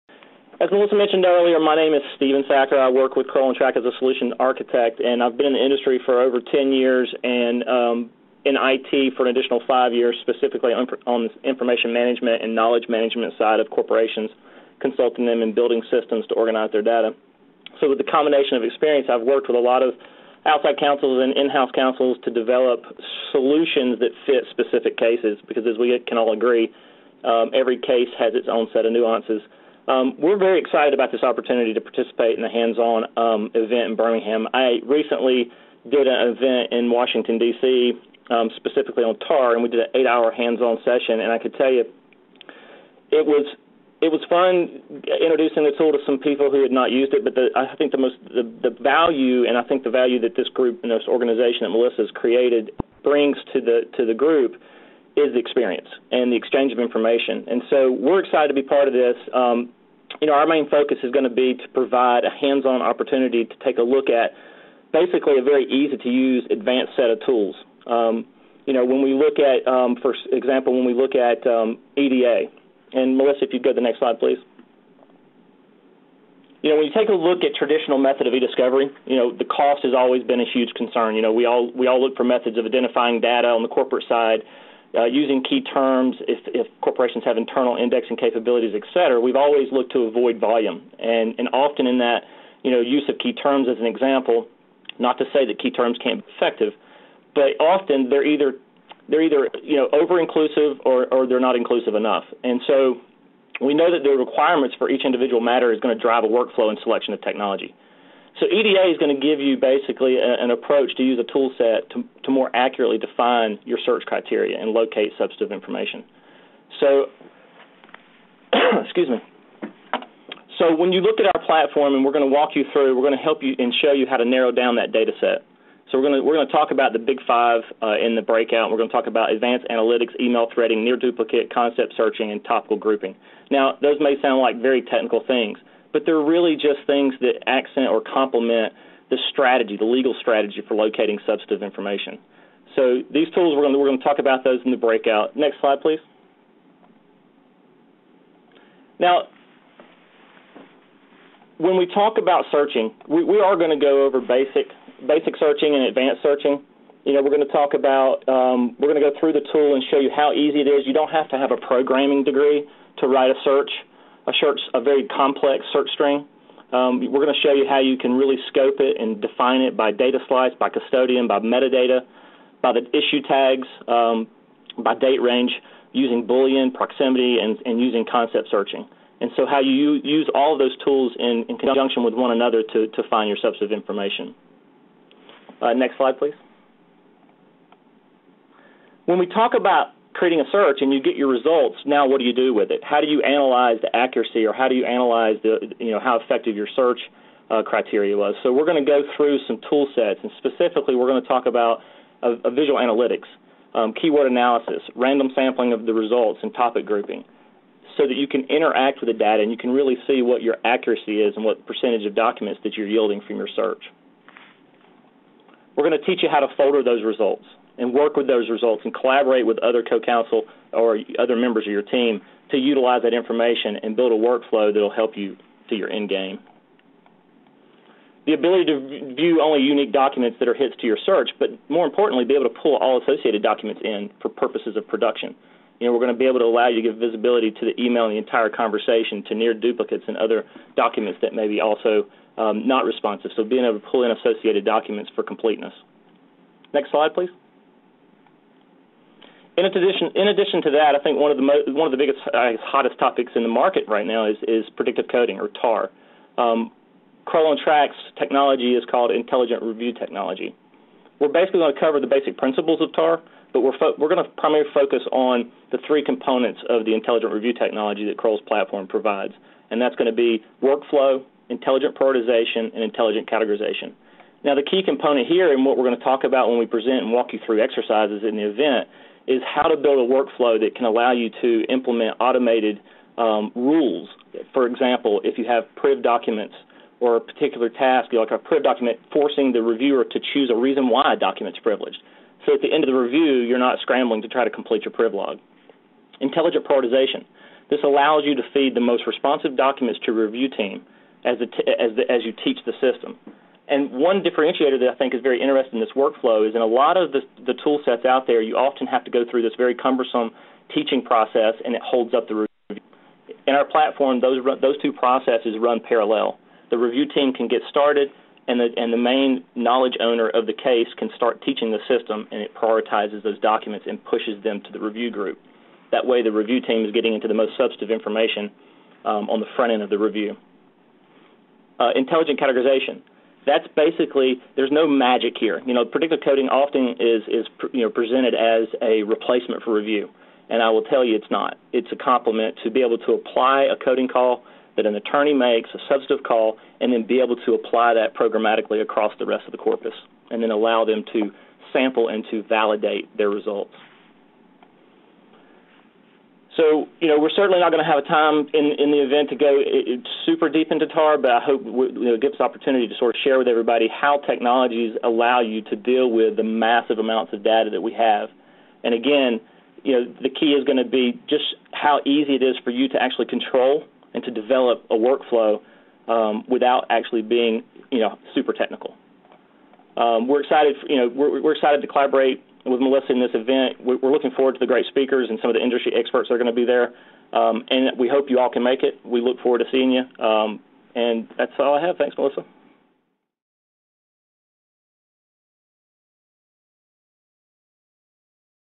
(ESI Roundtable, August 2013) HOT13 - Kroll Ontrack's TAR (Sneak Peek)
Stephen Thacker of Kroll OnTrack discusses their TAR solution for ESIRT's Hands-On Training & CLE Workshop (August 2013).